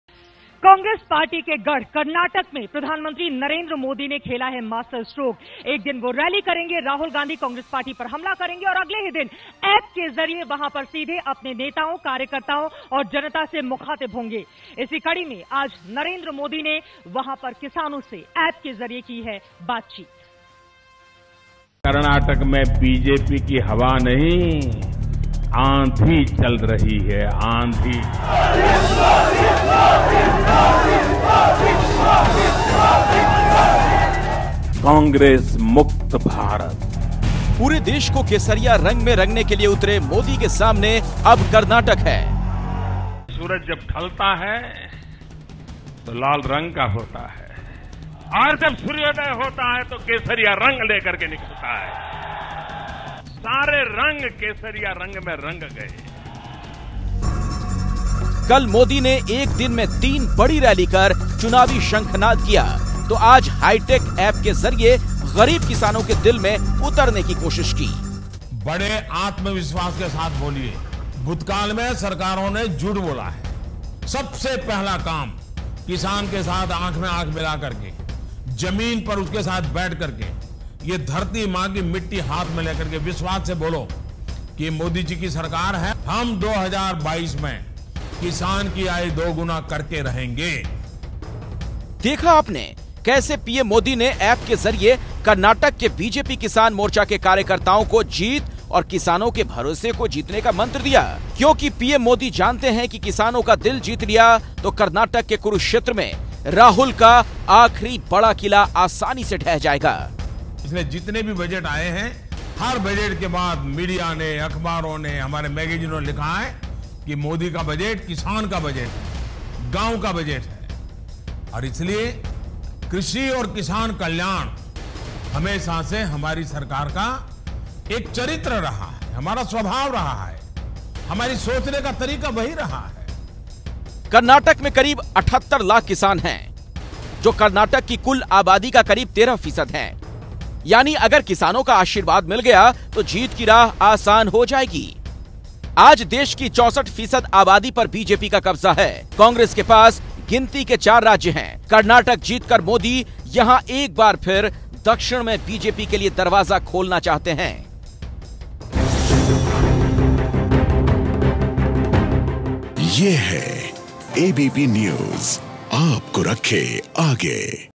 Congress के गढ़ में PM Modi ने चला बड़ा दांव | ABP News Hindi
PM Modi कर्नाटक चुनाव में नई रणनीति के साथ उतरे हैं . वो एक दिन रैली करेंगे . Rahul और Congress की सरकार पर हमला बोलेंगे. दूसरे दिन ऐप के जरिए सीधे कार्यकर्ताओं और जनता से बात करेंगे .